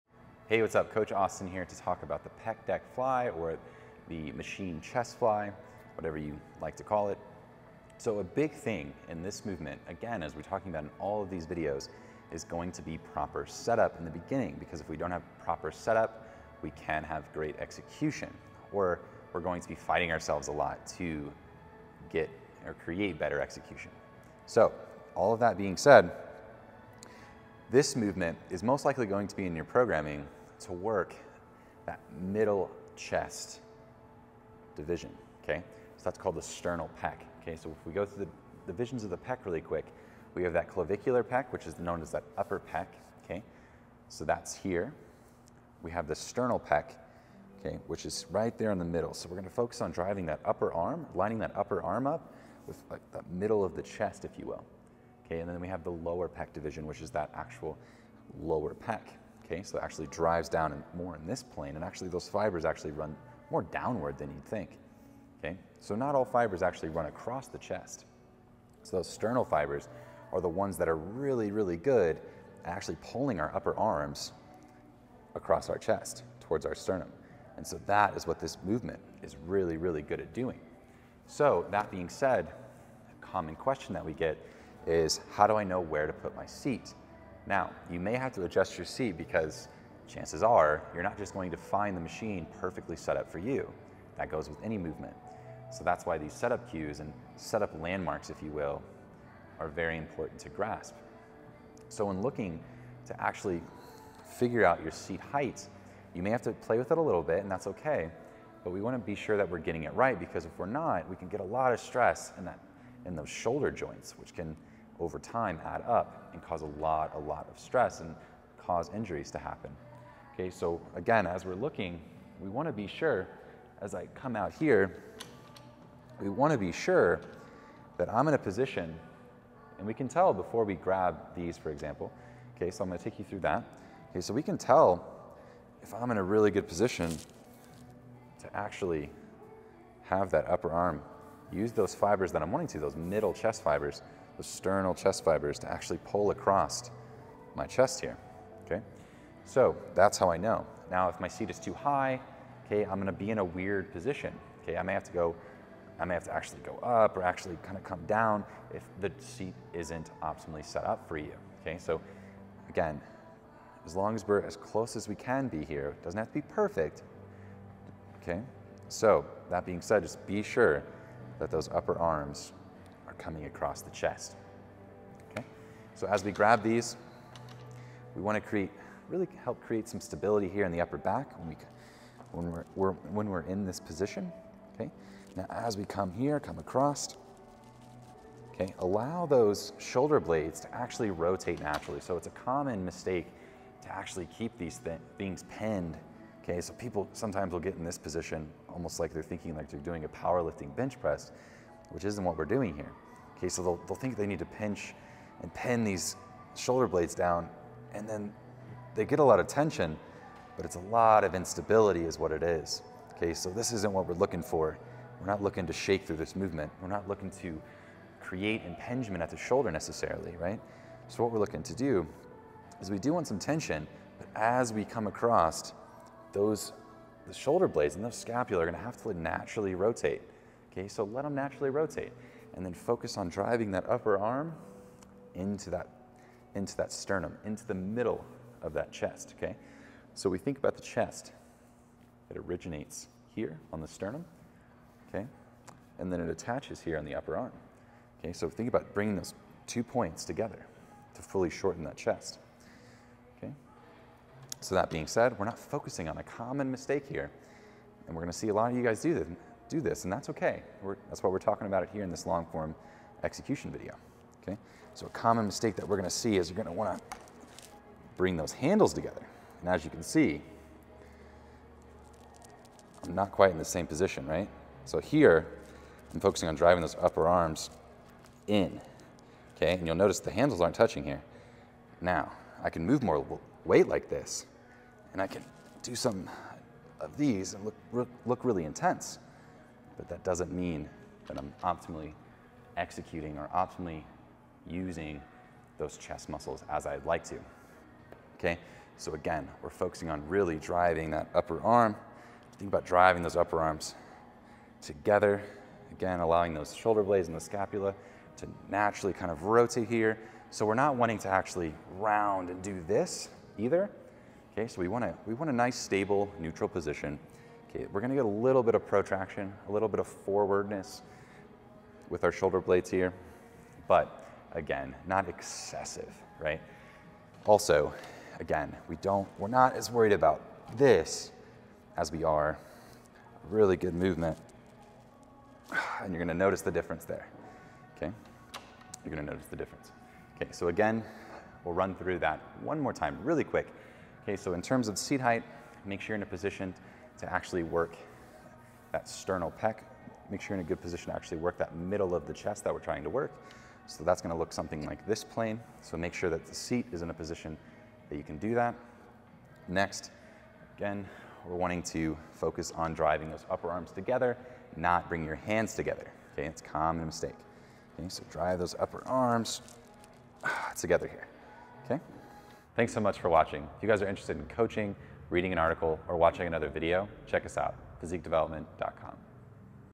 How to Seated Machine (Pec Deck) Chest Fly
In this video, Physique Development Coach, Austin, takes you through the proper technique for the seated machine chest fly — also known as the pec deck.

This is a great exercise for training the chest.

Timestamps:
(0:00) Intro
(0:34) Anatomy Used in Machine Chest Fly
(1:20) How to Properly Setup for Machine Chest Fly
(2:57) Proper Technique for Machine Chest Fly
(5:40) Recap on Proper Technique for Machine Chest Fly